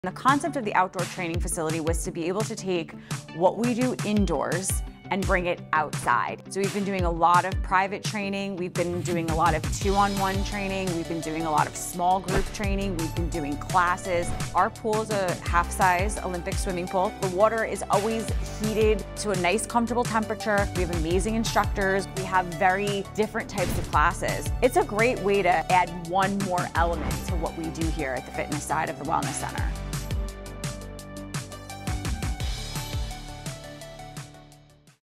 Vineyards Wellness Center Outdoor Gym & Acqua Classes
One of the only outdoor gyms at a country club in Naples. This outdoor gym is widely used by our members. Tune in to hear why we added this feature to our already incredible fitness center! We also offer aqua classes in our saltwater half-size olympic pool.